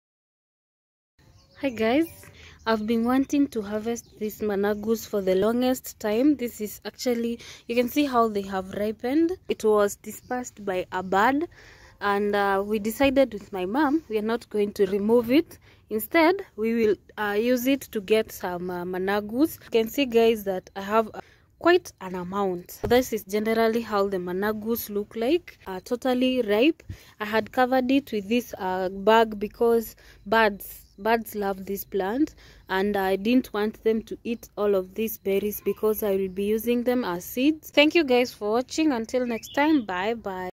Managu Farming in Kenya. HARVESTING MANAGU SEEDS TO REGROW FOR OUR HOME GARDEN
Managu farming is a low-cost business venture. these are managu that gew in our compound and we are harvesting to get more seeds and increase our farming activity. The managu takes at least two months to mature and you harvest for 4-8 weeks. Check this video for details